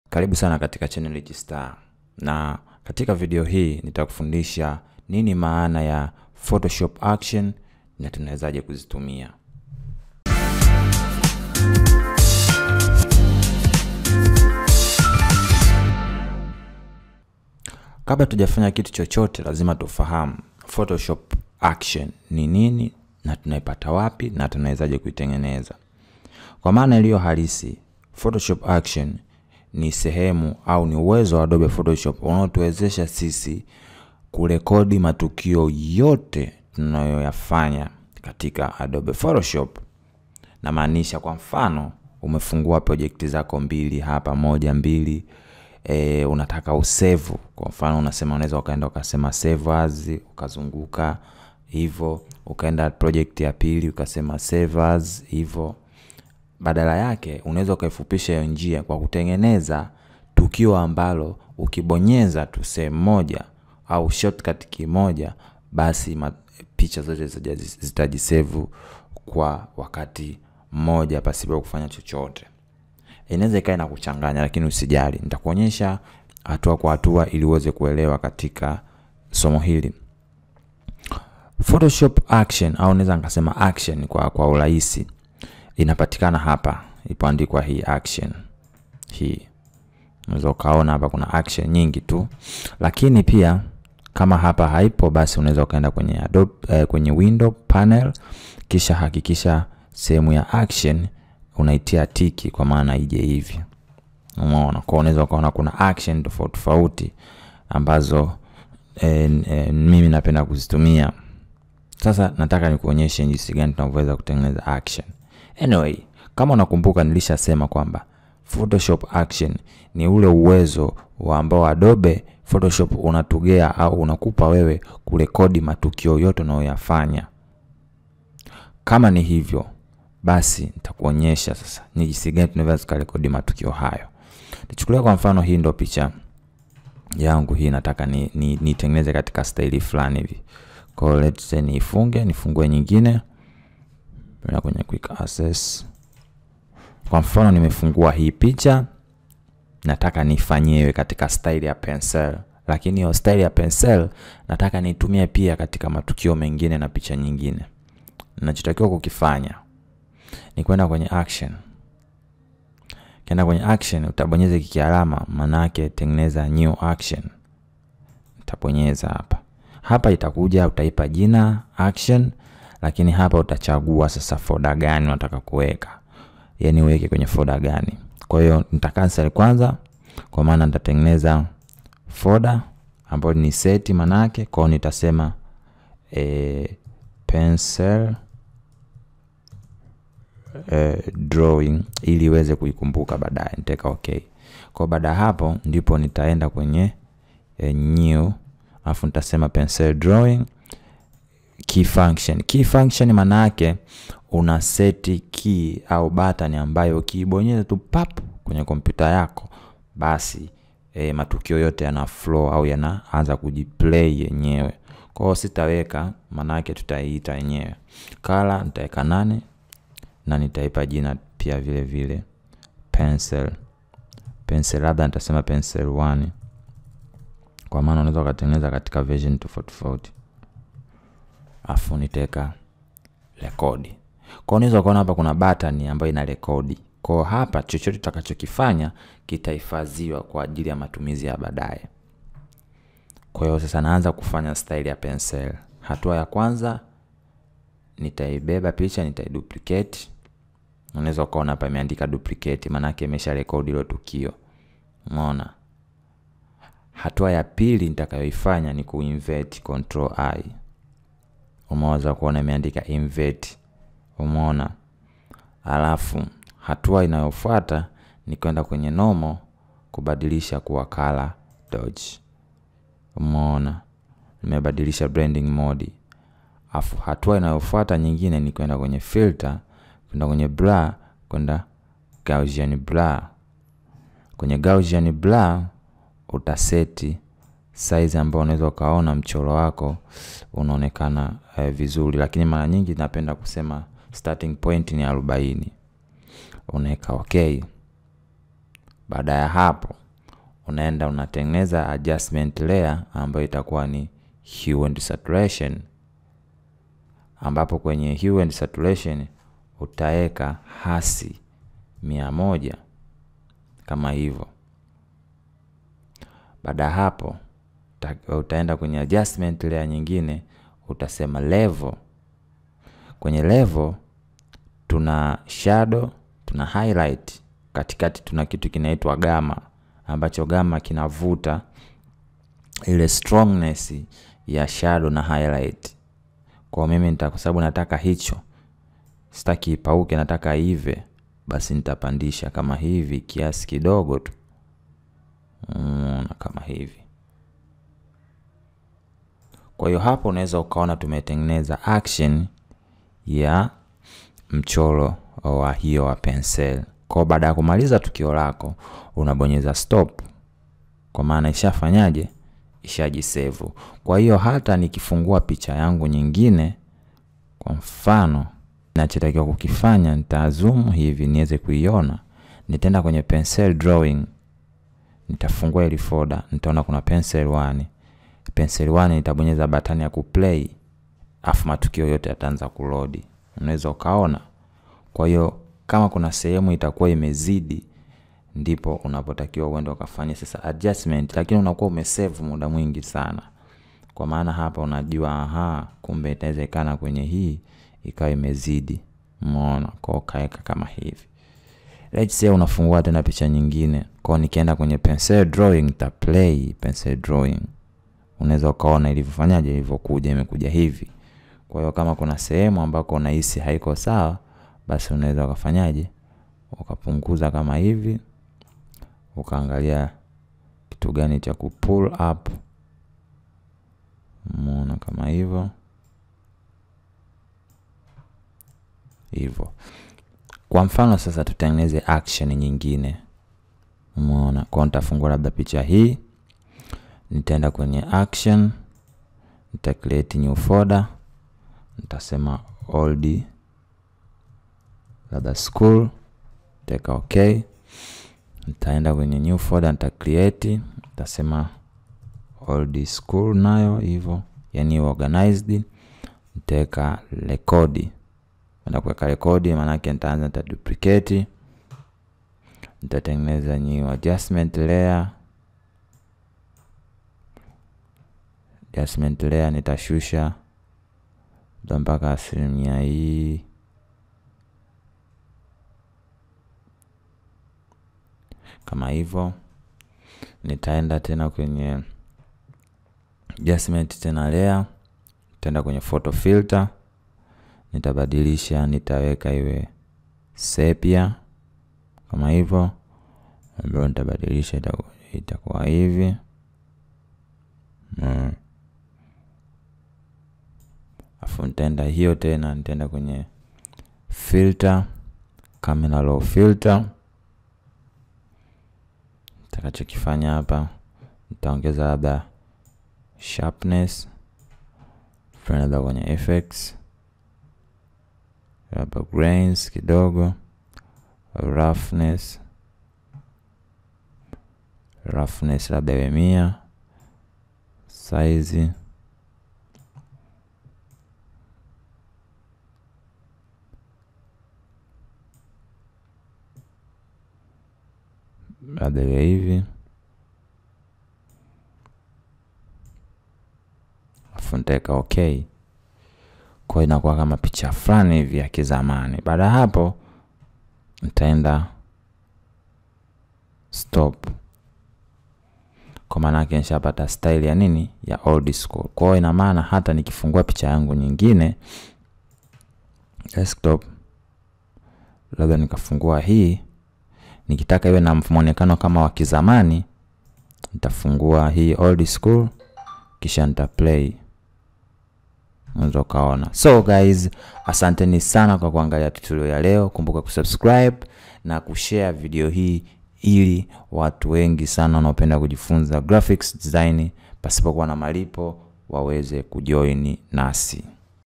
Fahamu zaidi kuhusu Photoshop ACTIONS
Katika Video Hii itajifunza na Fahamu zaidi kuhusu Photoshop ACTIONS, Jinsi ya kuzitengeneza, Kuziandaa na Kuzitumia kwa lengo la Kurahisisha kazi zako za Graphics